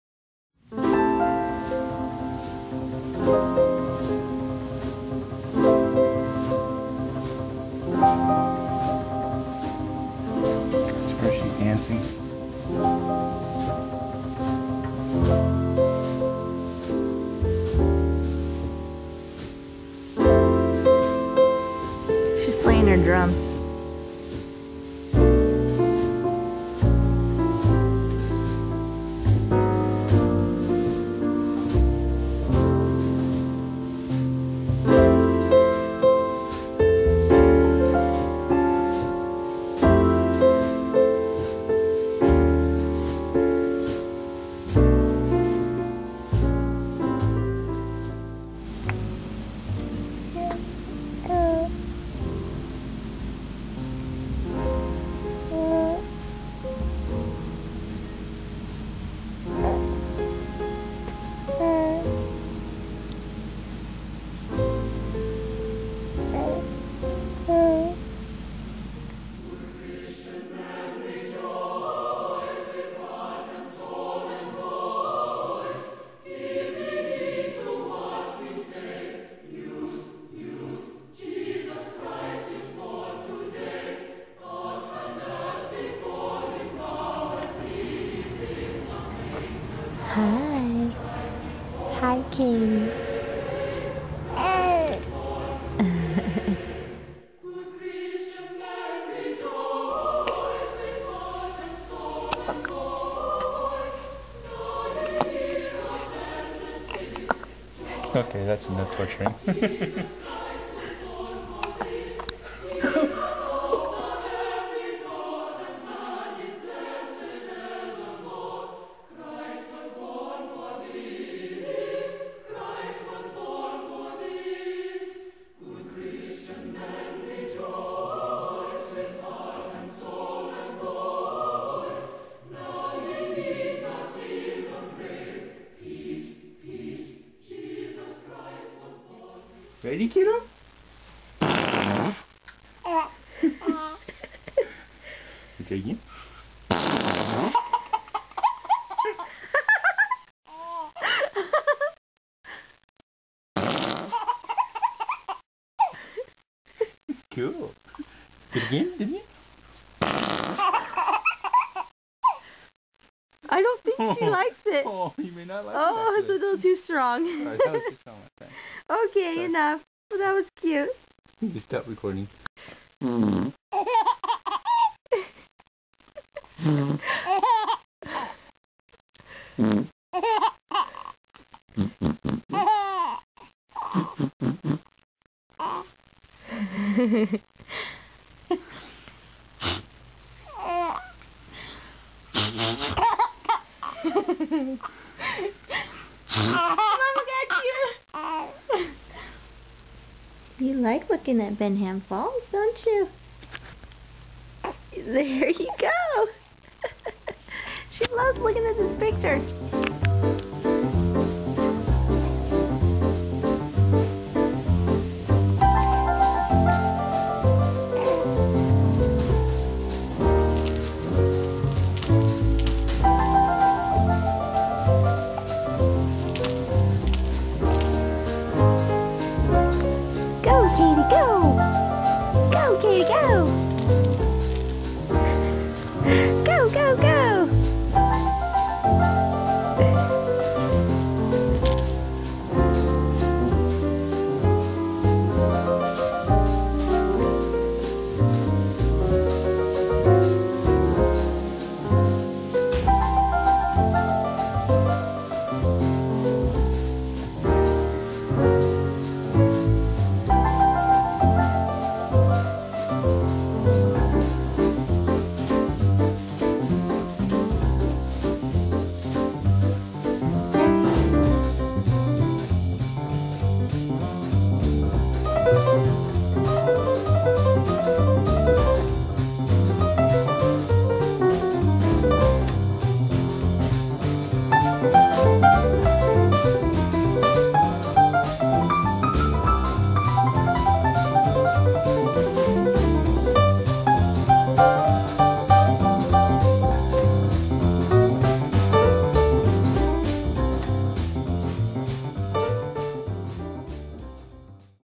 Kadie's 3rd Month
A video snapshot of Kadie's 3rd month of life